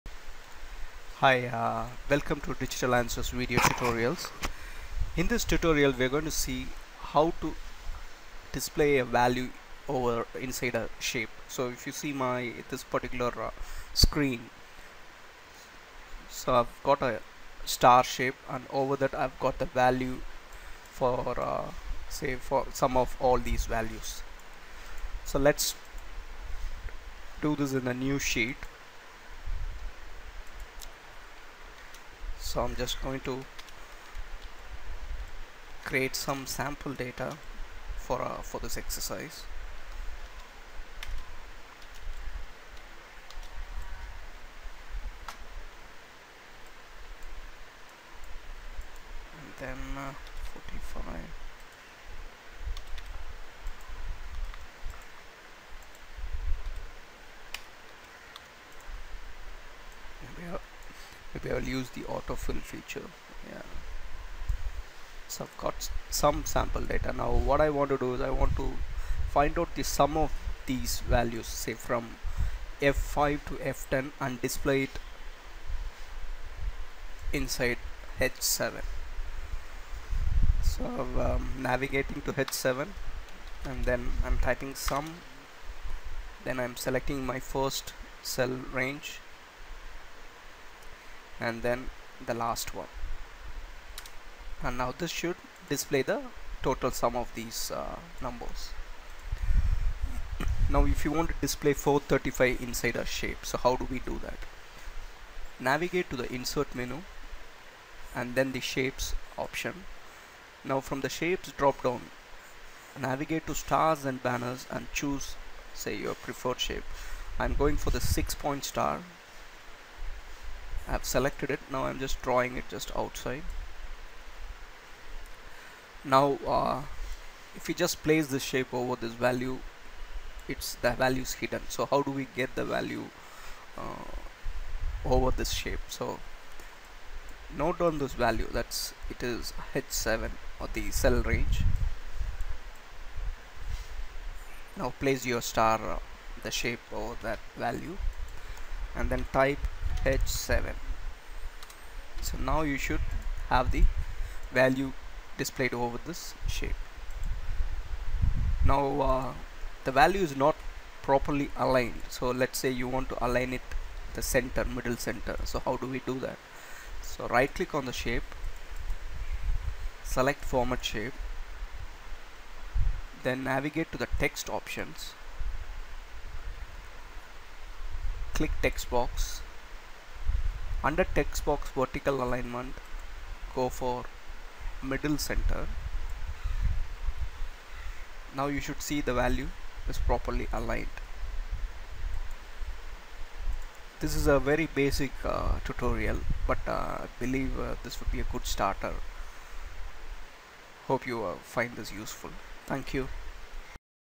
Insert and display value over the shape in Excel 2013
This video tutorial provides the steps for inserting a shape and displaying value on that shape in Excel 2013